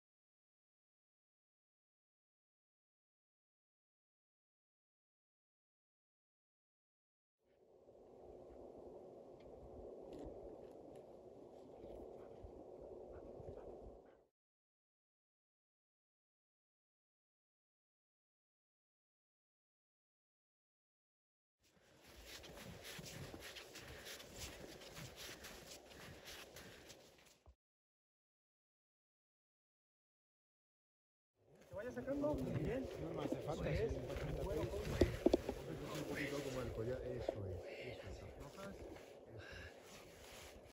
Perros de búsqueda de personas
En Panticosa la Guardia Civil lleva a cabo el plan de instrucción de perros de búsqueda y rescate en avalanchas.